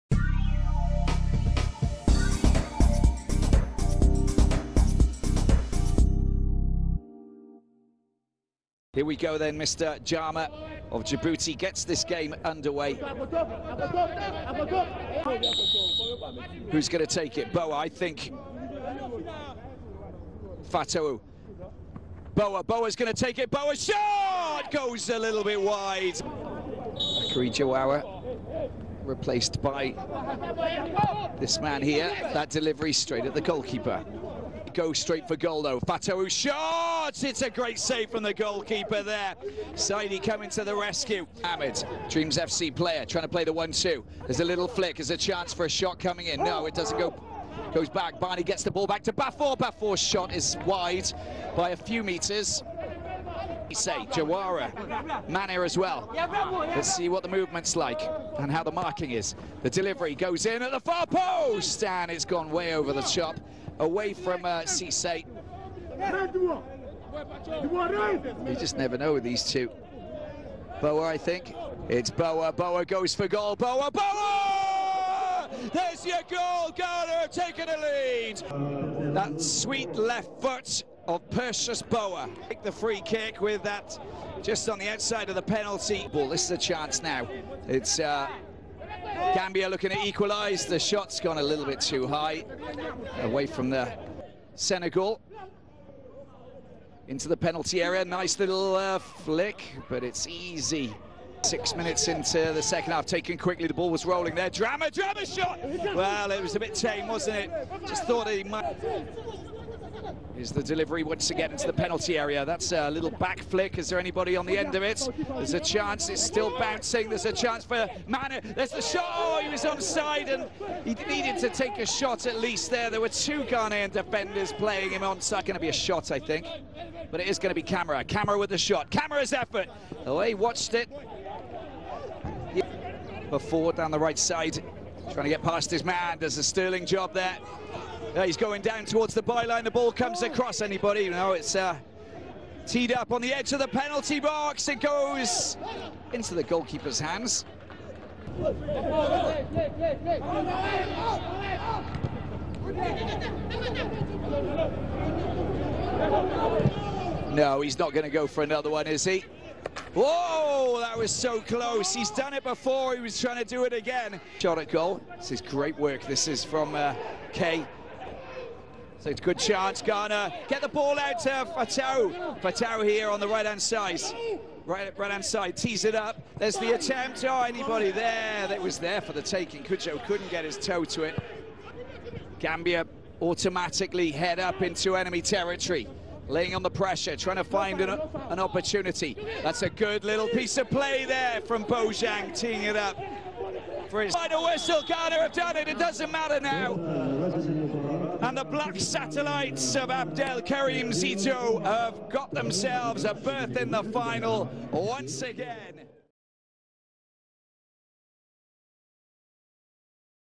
Africa U-20: Ghana vs Gambia 1 - 0, Semi final. Goal and Highlights
Africa U-20 Cup of Nations 2021 Semi final, Ghana vs Gambia. Highlights. Semi final.